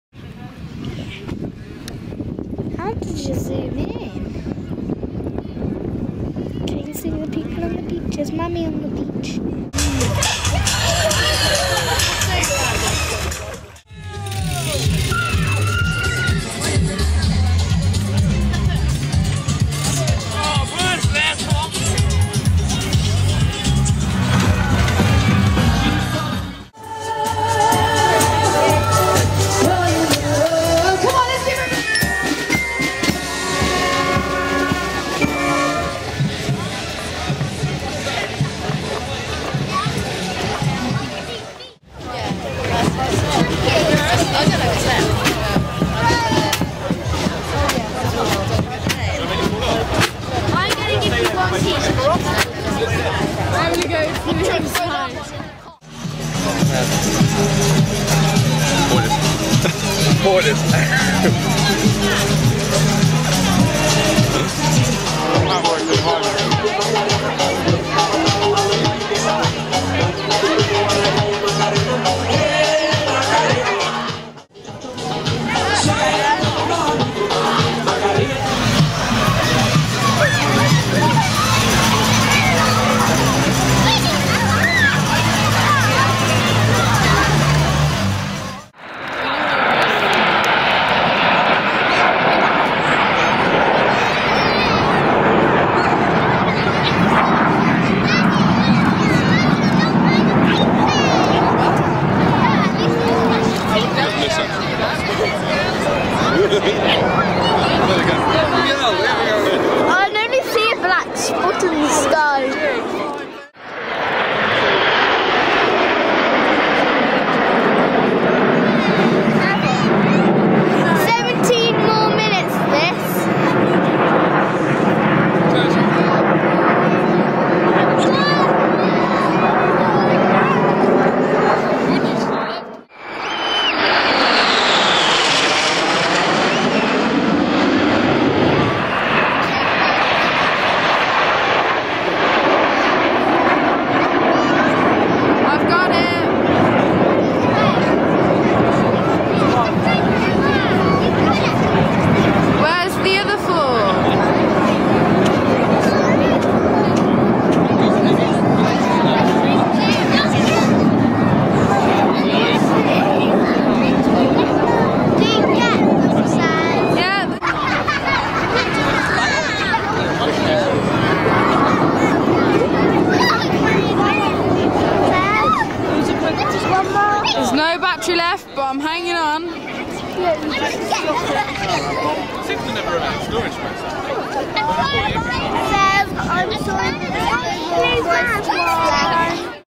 Swanage Carnival 2011
Video pieced together quickly that was shot by Becky and the children at Swanage Carnival 2011.